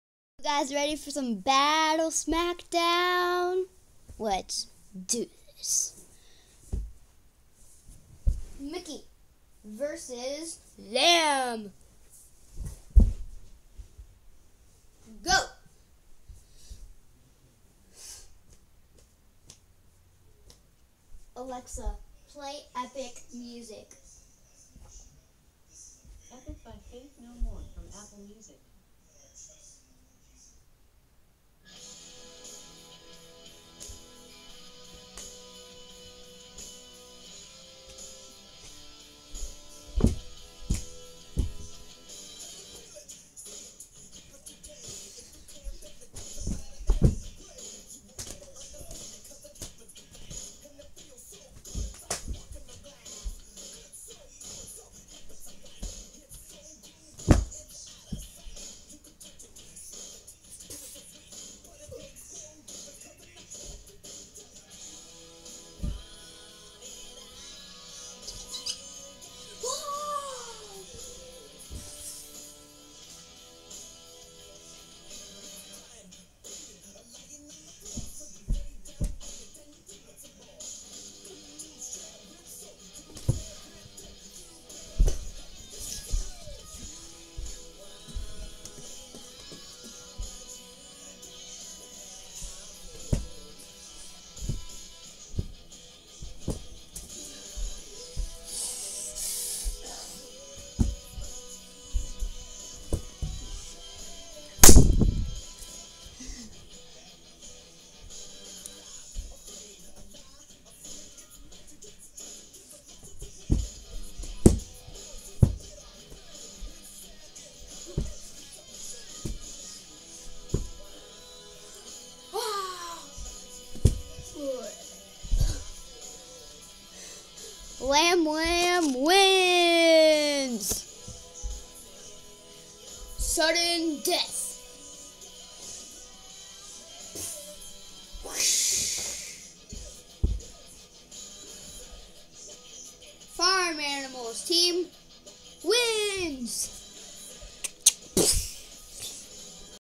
Battle Smackdown #1
Micky and sheep fight in an epic battle!  Who will win??